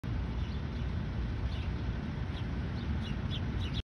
AAA Trucking LLC International LT Pt: 4 on 7/1/23
AAA Trucking LLC International LT Pt: 4 in DC. AAA. Trucking. AAA Trucking. AAA Trucking LLC. Truck is coming out of Norristown Pennsylvania. It is making deliveries. It is resting. Truck is is heading towards Baltimore/Norristown Pennsylvania. Enjoy the video!